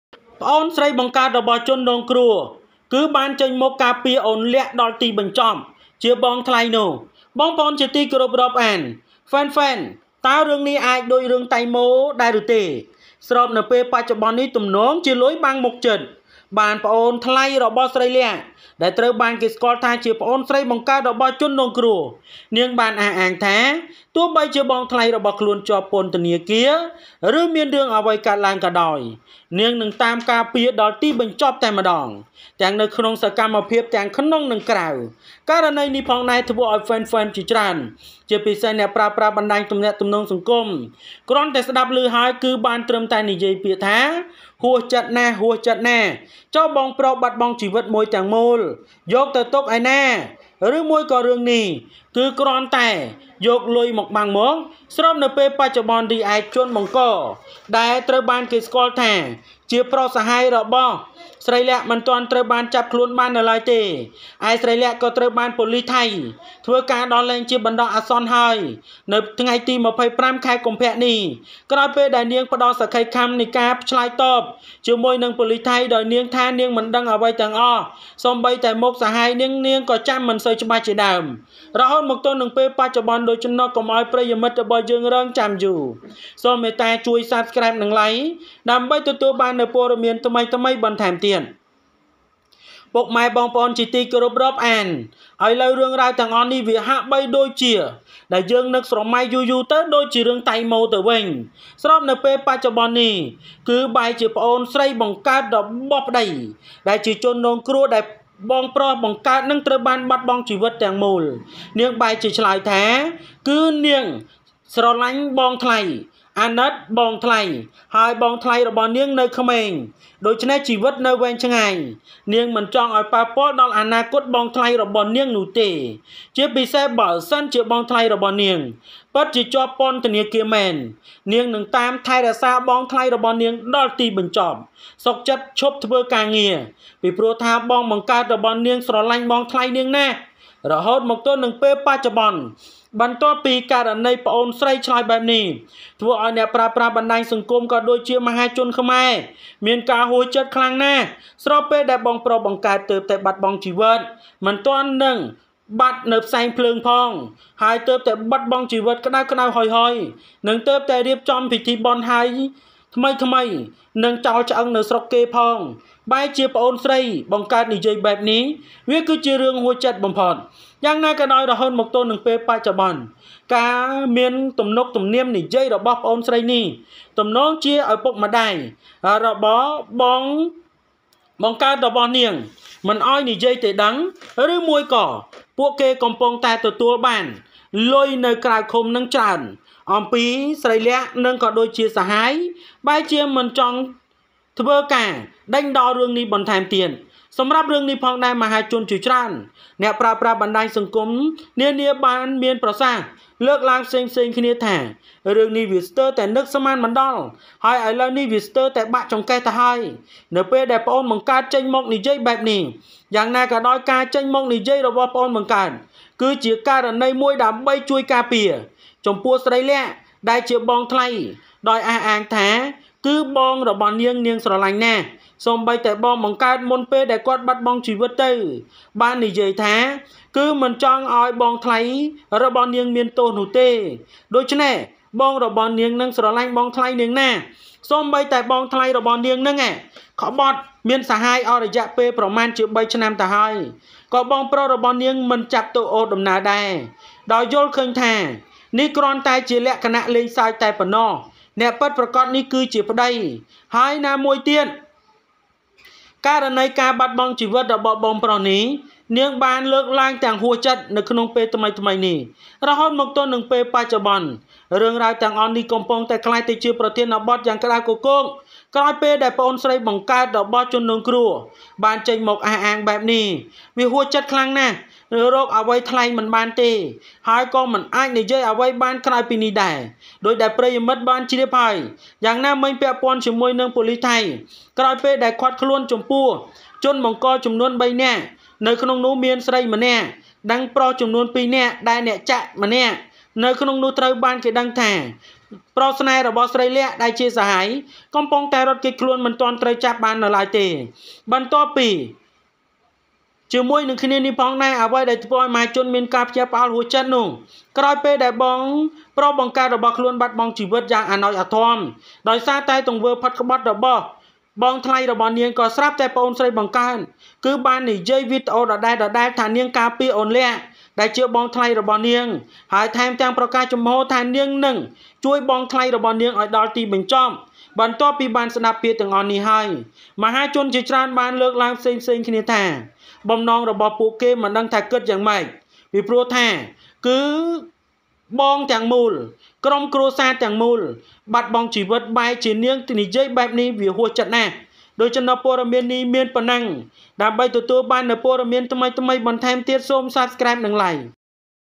ប្អូនស្រីបង្កើតរបស់ជនរងគ្រោះ ចេញមុខការពារ Oun Leakដល់ទីបញ្ចប់
ប្អូនស្រីបង្កើតរបស់ជនរងគ្រោះ ចេញមុខការពារ Oun Leakដល់ទីបញ្ចប់ The victim's sister came out to defend Oun Leak to the end.